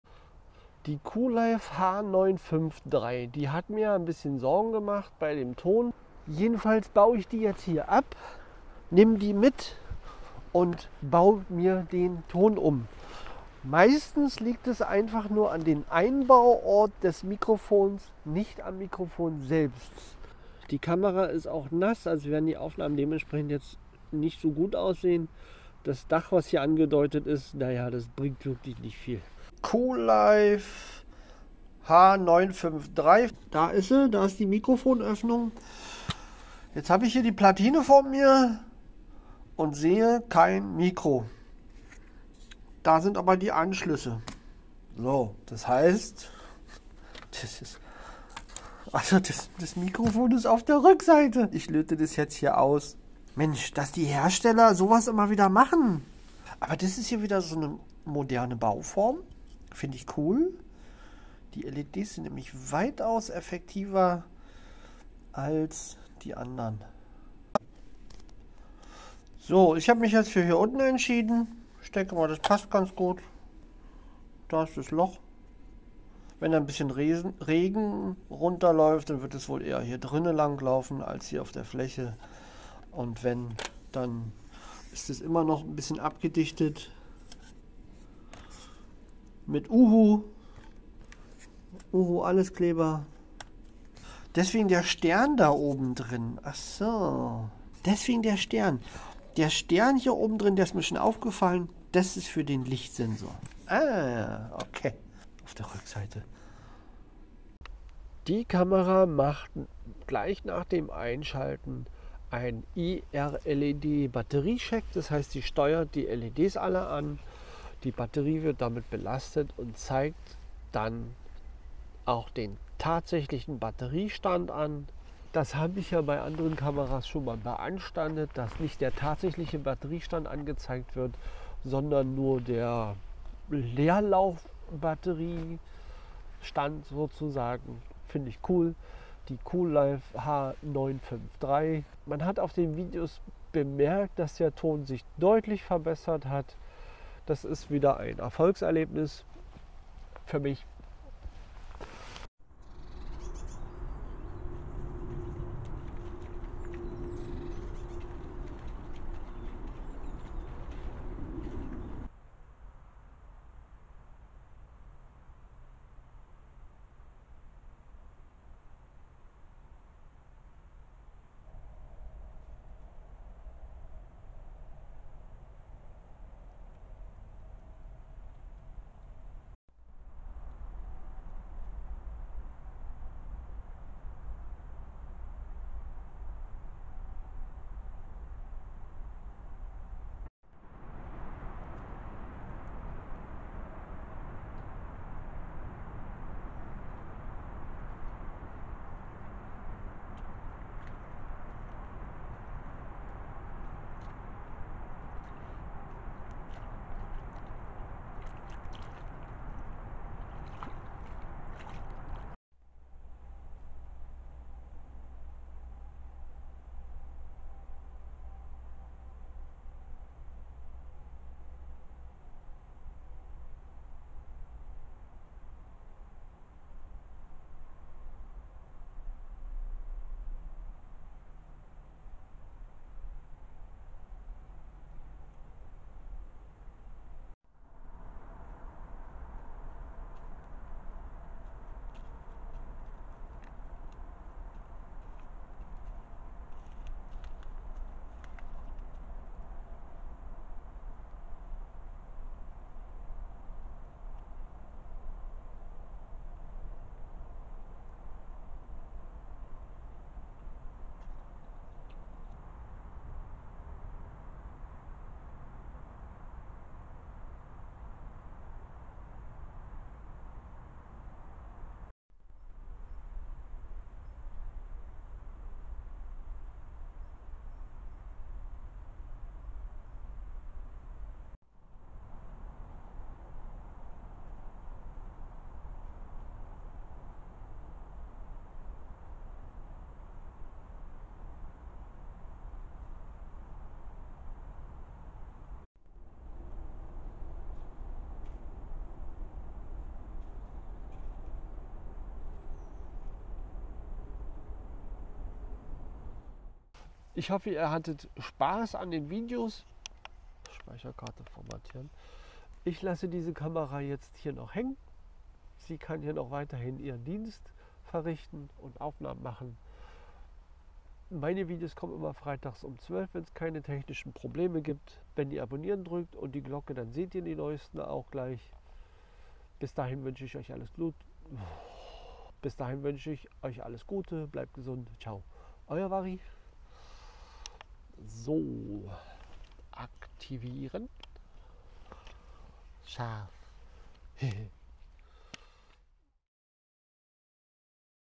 Coolife H953 Mikrofon Umgebaut
Die Coolife H953
Mikrofon umgebaut und schon ist die Kamera eine bessere ☺